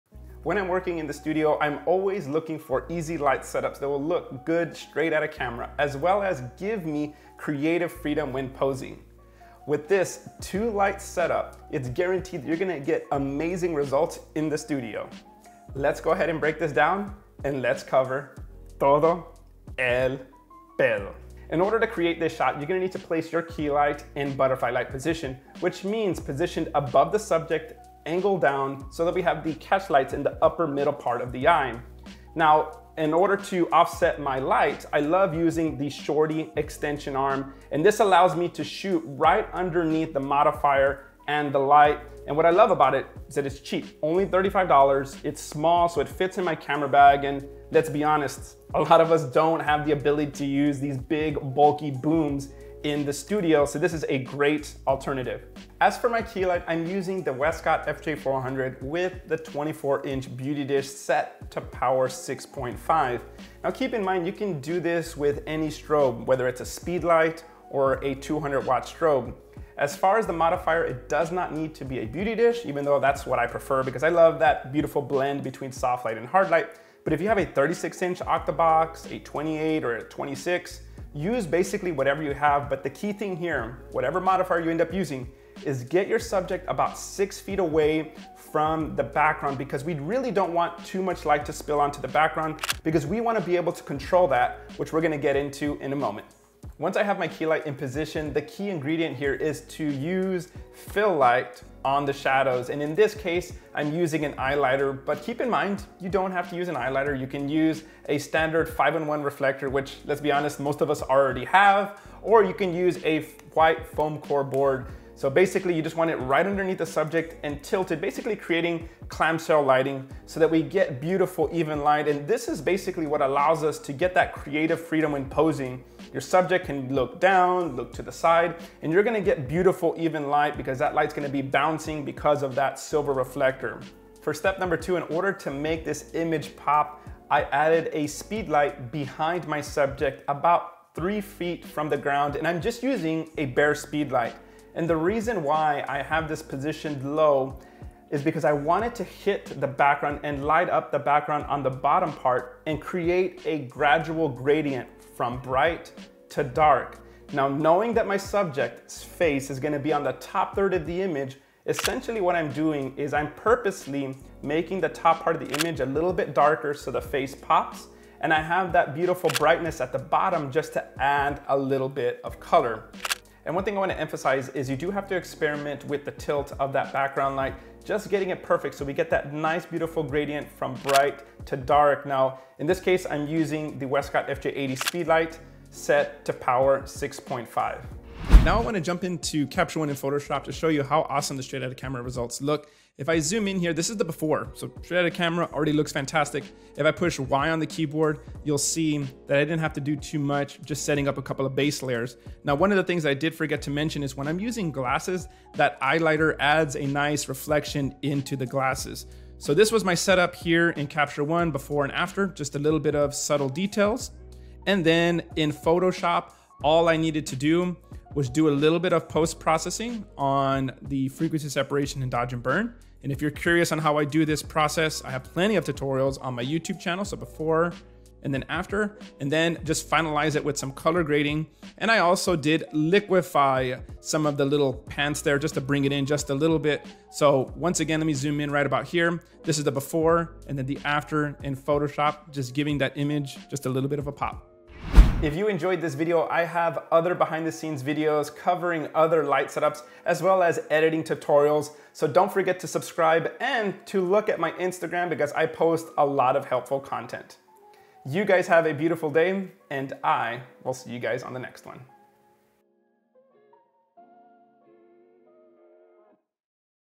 The PERFECT Two Light Setup for Studio Portraits
This video breaks down my perfect two-light setup for studio portraits. Here I use Westcott FJ400 and the Joel Grimes 24" Beauty Dish for amazing images. Big thanks to my friend Marco & Lucas for recording the BTS video footage.

Canon RF 24-105